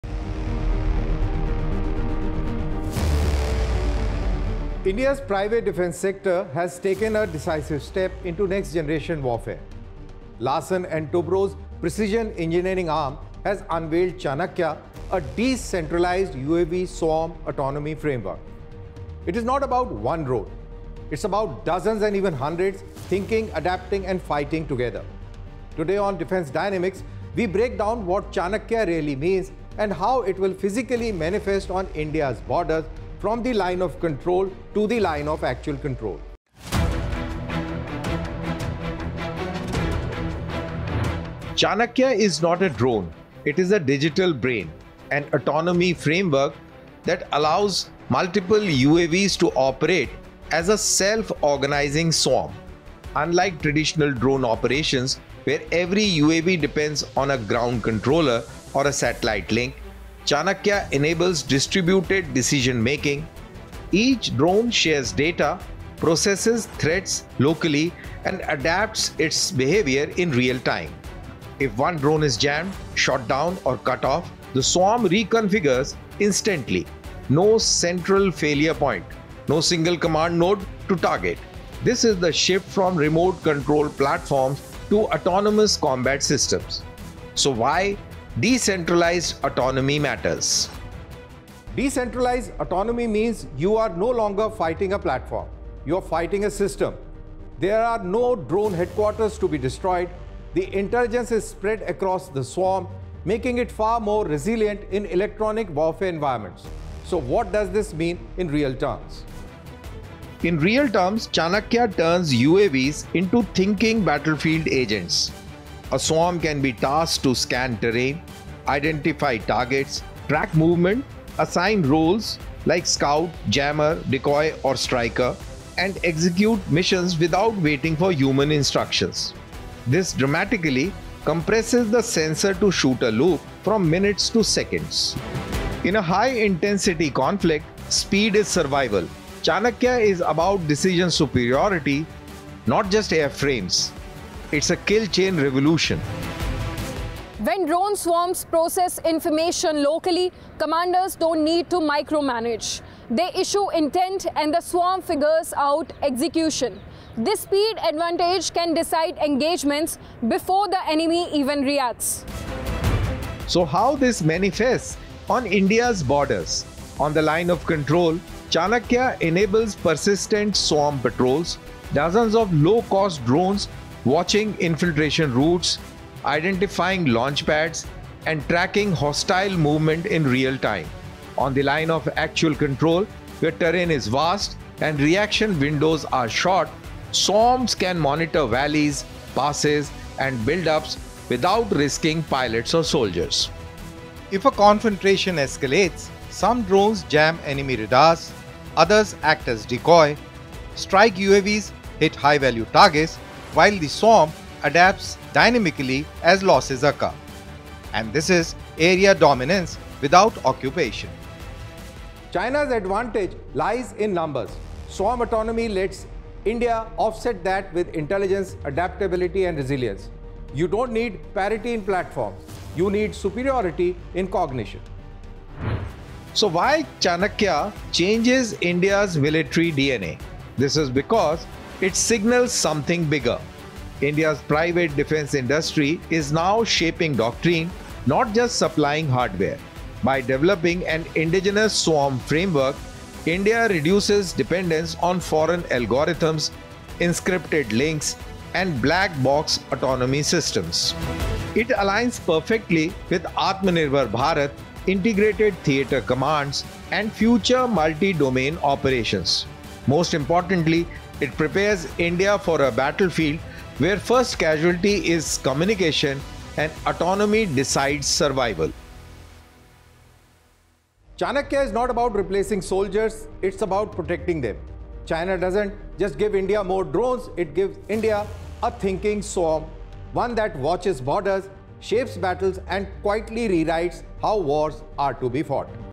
'Chanakya': India's AI Drone Swarm | Defence Dynamics
CHANAKYA is not a drone; it is a digital brain. This video explains how India’s indigenous swarm autonomy framework marks a decisive shift from remotely controlled UAVs to decentralised, AI-driven combat systems. Unlike traditional drones that rely on vulnerable ground links, CHANAKYA enables UAV swarms to sense, decide, and adapt in real time, even under jamming or attack. We break down how this technology compresses the sensor-to-shooter loop, delivers decision superiority, and enables persistent dominance along the Line of Control and Line of Actual Control. More importantly, CHANAKYA signals a deeper transformation in India’s military DNA, where private industry shapes doctrine and autonomy replaces communication-dependent warfare. In future battlefields, survival will belong to forces that can think, adapt, and fight without waiting for instructions.